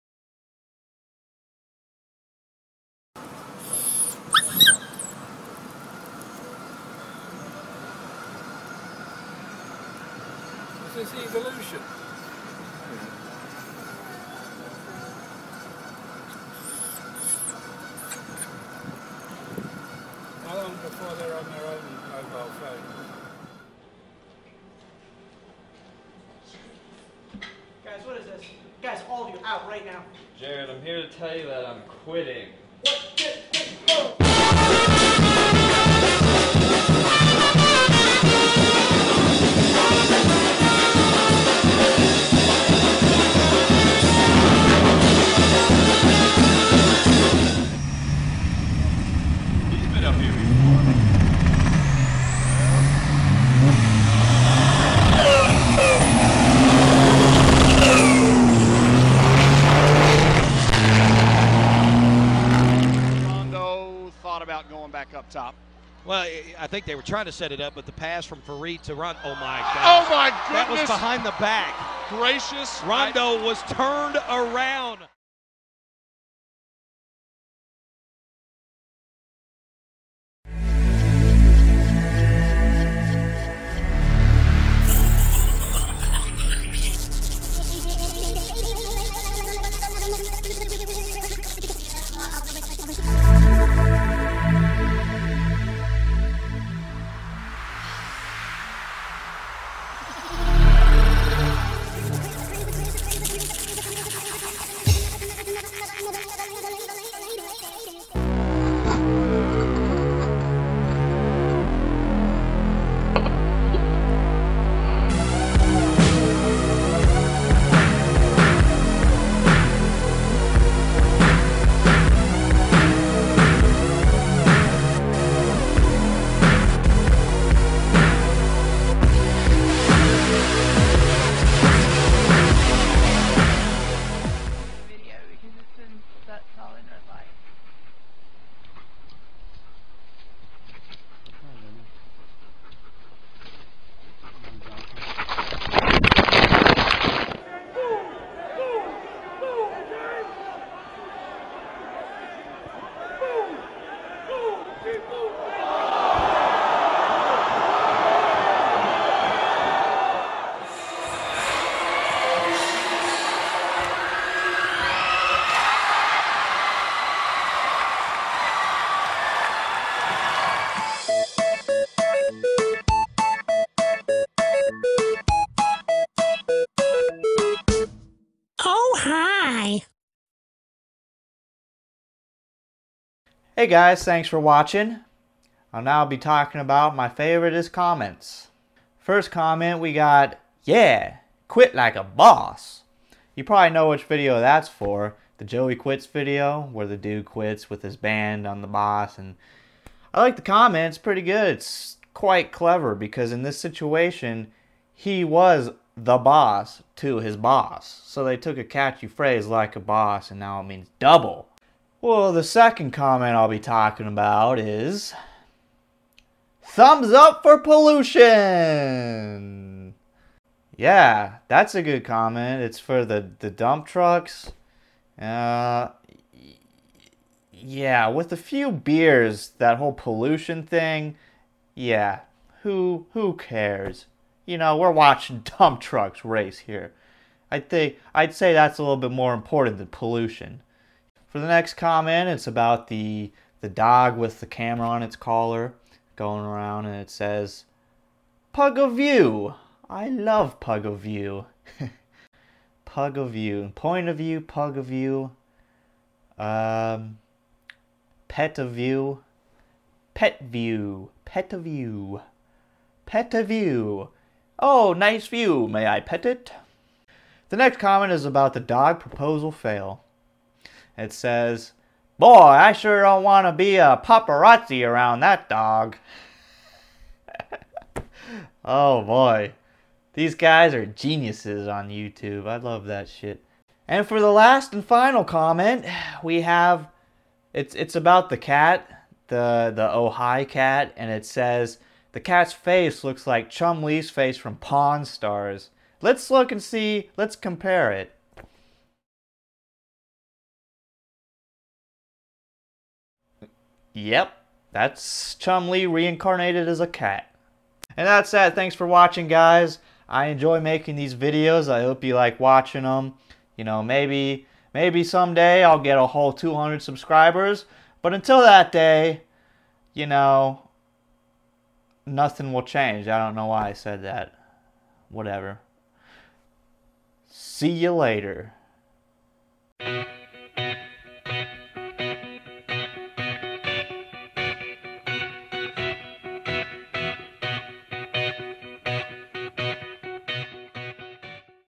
Viral Vids 10-19-11 Dog Ball
This episode includes a) clever dog plays fetch b) joey quits c) dump truck racing d) no look alley-oop e) burning man time lapse f) pug pov camera g) dog ruins proposal h) kimb slice again i) dunk over mom j) oh hai cat